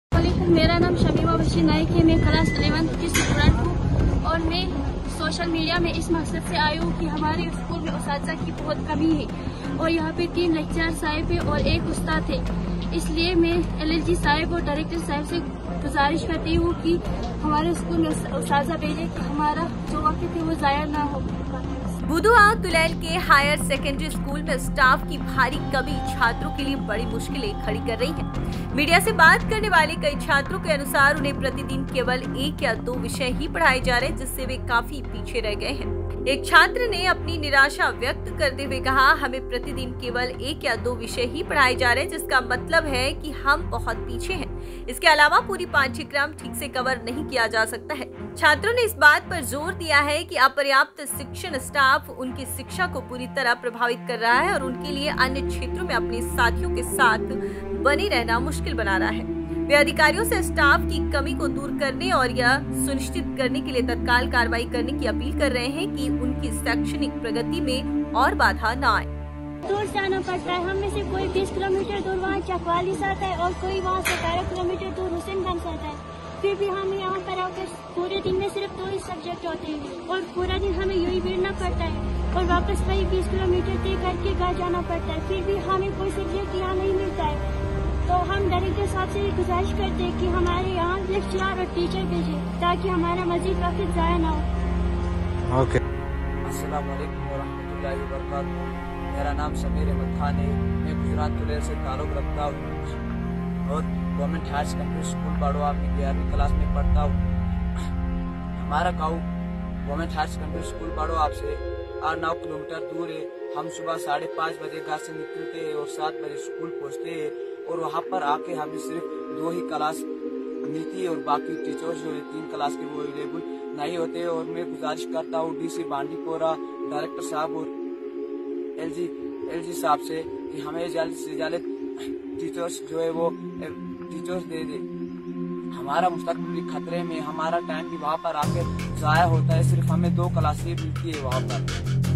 Buduab तुलैल हायर सेकेंडरी School में छात्रों के लिए बनी बड़ी चुनौती , छात्रों का भविष्य खतरे में !
बुदुआब तुलैल हायर सेकेंडरी स्कूल में स्टाफ की गंभीर कमी छात्रों के लिए गंभीर मुश्किलें उत्पन्न कर रही है। कई छात्रों ने मीडिया से बात करते हुए बताया कि उन्हें प्रतिदिन केवल एक या दो विषय ही पढ़ाए जा रहे हैं, जिसके कारण वे अपनी पढ़ाई में काफी पीछे रह गए हैं।
एक छात्र ने अपनी निराशा व्यक्त करते हुए कहा, "हमें प्रतिदिन केवल एक या दो विषय ही पढ़ाए जा रहे हैं, जिसका मतलब है कि हम बहुत पीछे हैं। इसके अलावा, पूरा पाठ्यक्रम ठीक से कवर नहीं किया जा सकता है।"
छात्रों ने जोर देकर कहा कि अपर्याप्त शिक्षण स्टाफ उनकी शिक्षा को बुरी तरह प्रभावित कर रहा है और उनके लिए अन्य क्षेत्रों में अपने साथियों के साथ बने रहना मुश्किल बना रहा है। वे अधिकारियों से अनुरोध कर रहे हैं कि स्टाफ की कमी को दूर किया जाए और उनकी शैक्षणिक प्रगति में और बाधा न आए, इसके लिए तुरंत कदम उठाए जाएं।